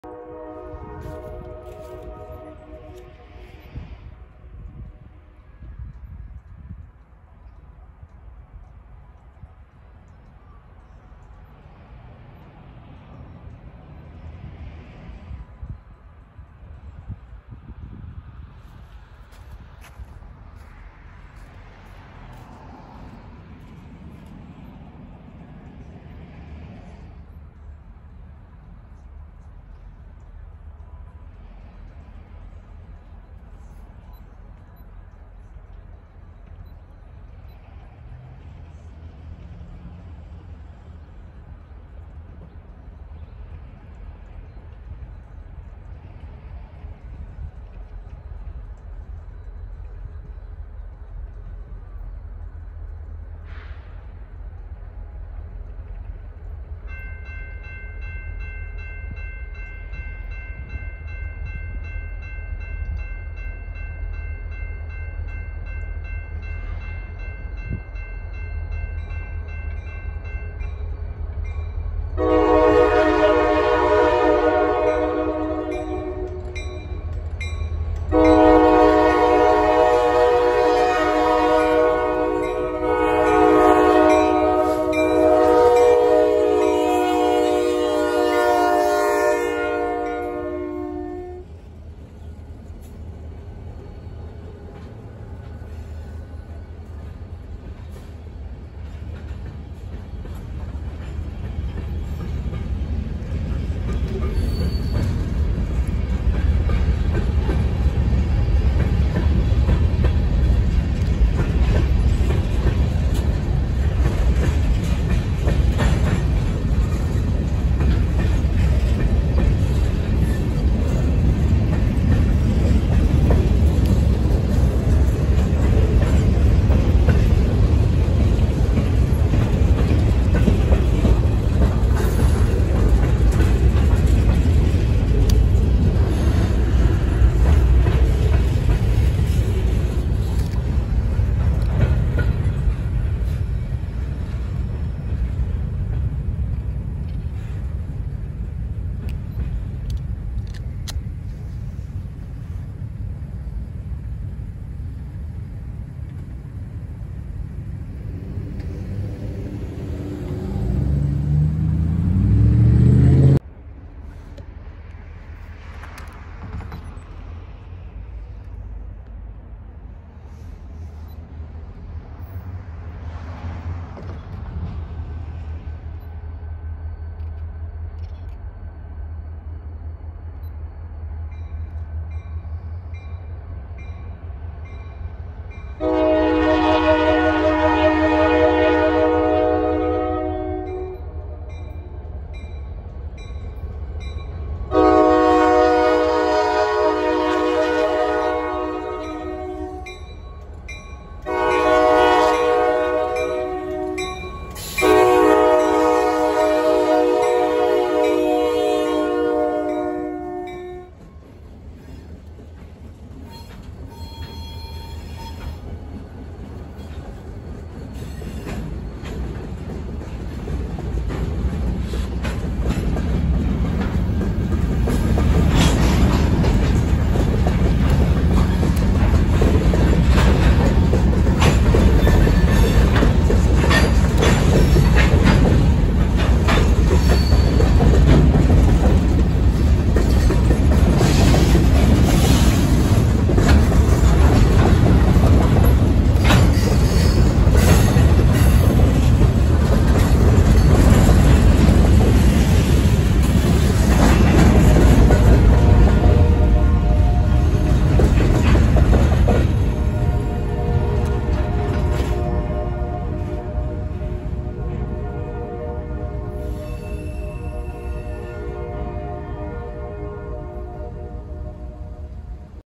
[4K 60fps] An Operation Lifesaver GP59 on the AVR
Southern speed queen slogs the Yankee local...
This was a quick edit using some new software. Thanks to Jason Capra, I received word that last Thursday's AVR-3 to Washington had the CLXX (former NS) GP59 4552 on a longer-than-usual consist. After waiting in Venetia for two hours, -3 came by with 4552 in the lead, and a pair of new GATX boxcars sublettered for LRS in the consist. I "chased" the train to another crossing further up the Venetia-Finleyville road that I had always wanted to shoot at.
Look for more 4K 60fps content in the coming weeks, and for photos, please check out Instagram.